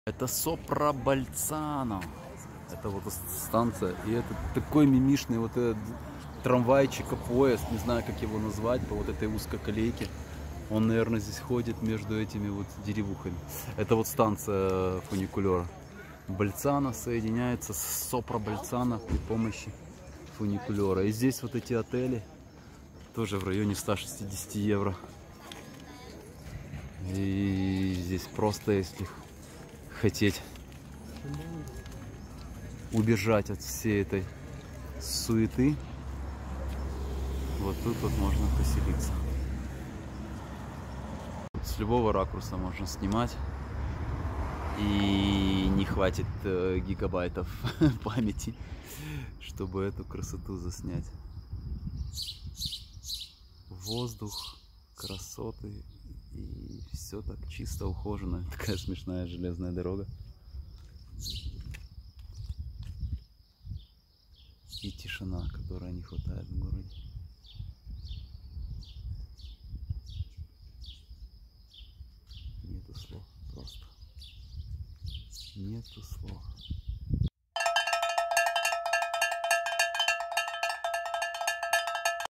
Лучше Больцано только Сопрабольцано😍
Сопрабольцано — старинное поселение, известное с 13 века. Оно было частью Тирольского графства. Сейчас это тихий горный курорт.

samtaimzaifil